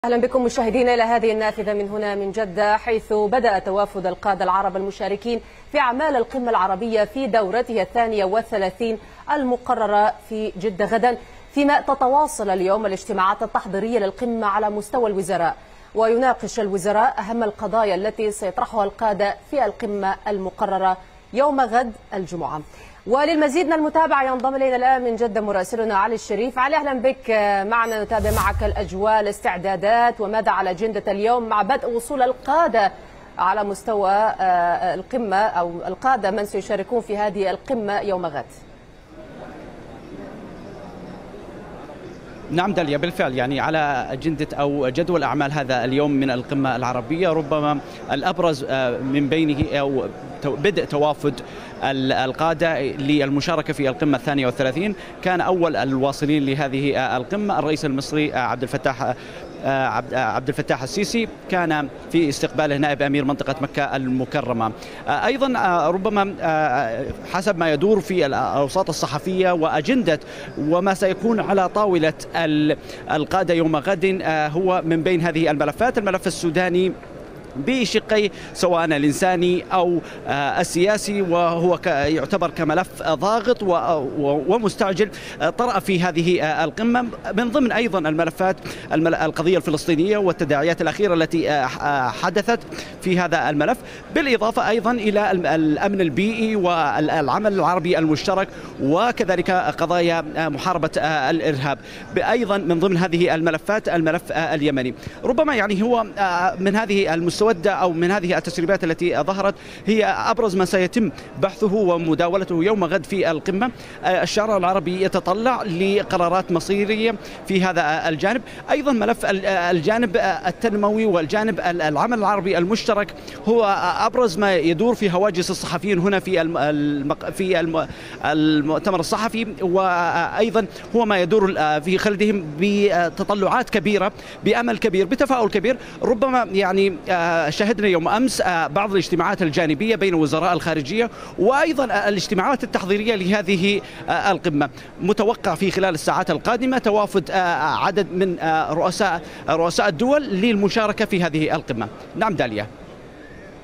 تواصل الاجتماعات التشاورية قبيل قمة القادة العرب
تواصل الاجتماعات واللقاءات التحضيرية لقمة جدة، وبدء توافد القادة العرب المشاركين في أعماله غدا.